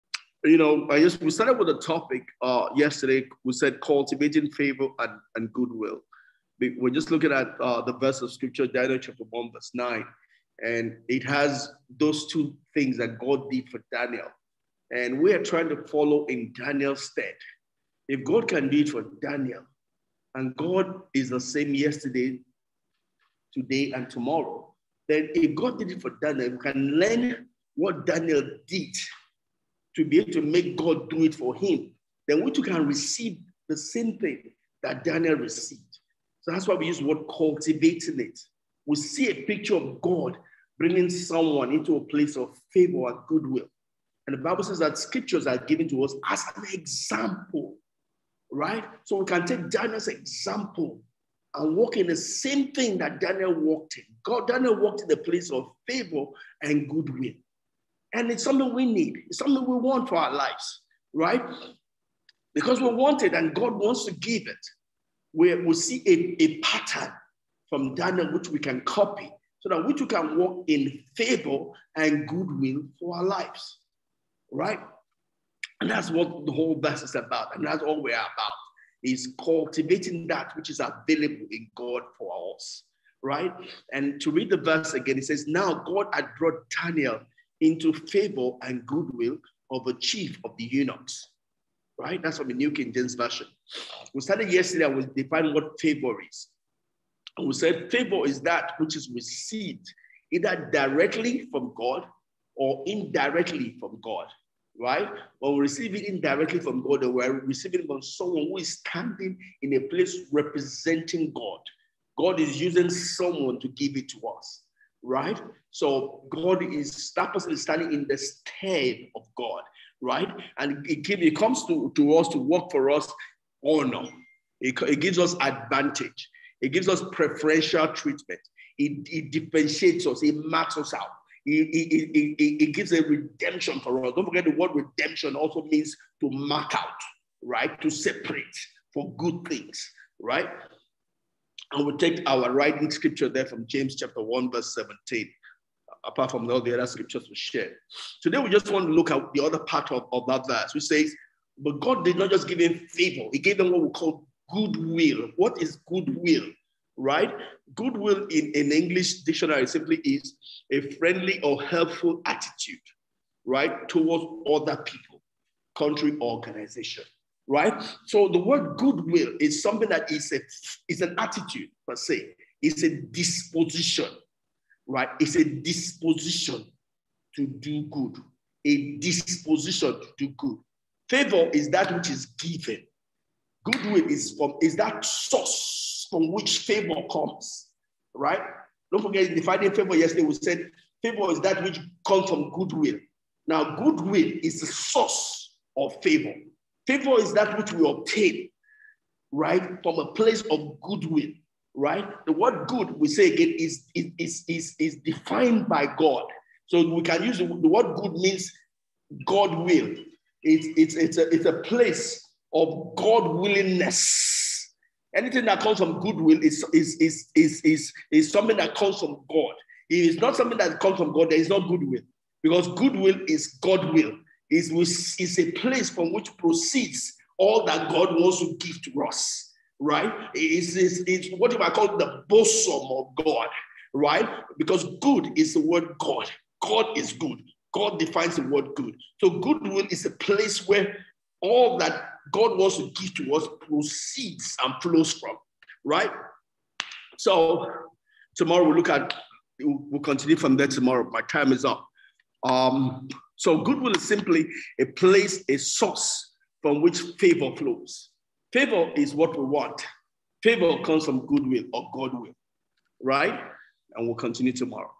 Favor and Goodwill | Part Four | The Saint.
This is an extract from our weekly weekday prayer meeting.

Topic: Favor and Goodwill.